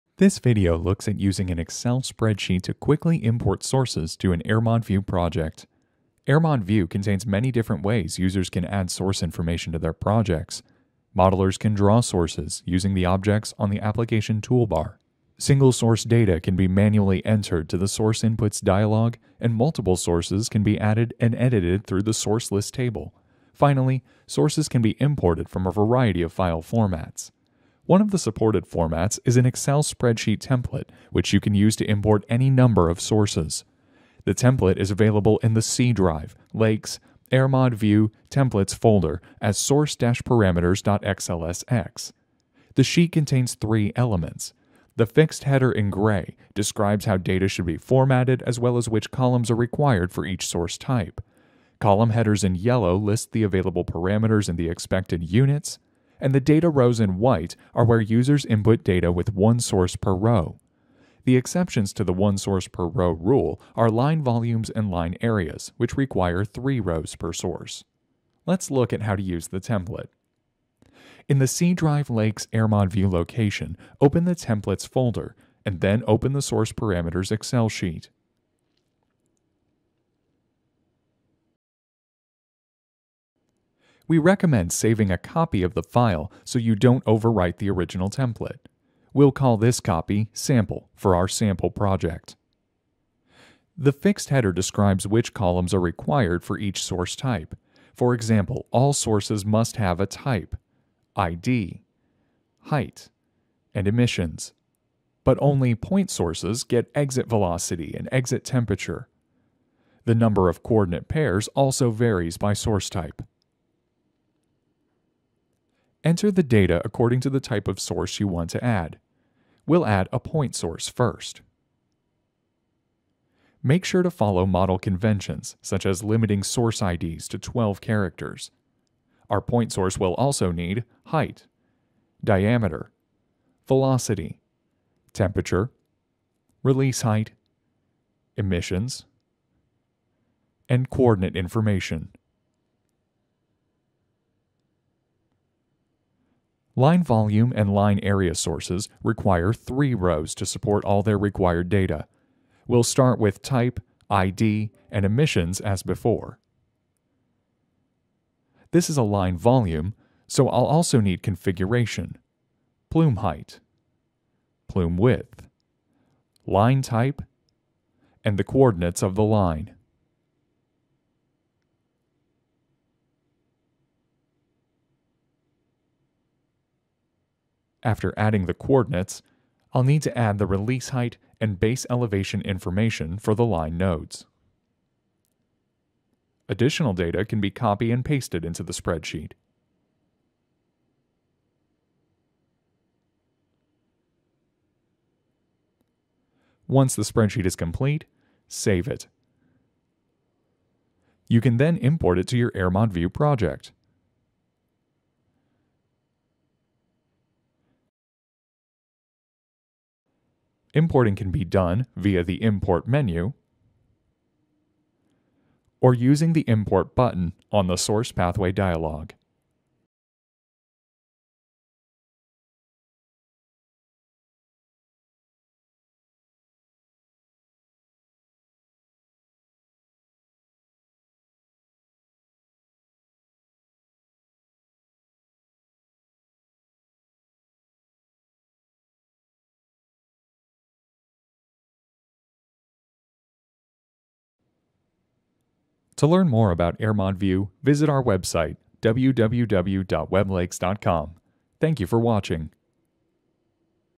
AERMOD View Import Sources from Excel
There are several ways to import sources into your AERMOD View project. This video reviews use of the Excel spreadsheet template to make importing multiple sources easy!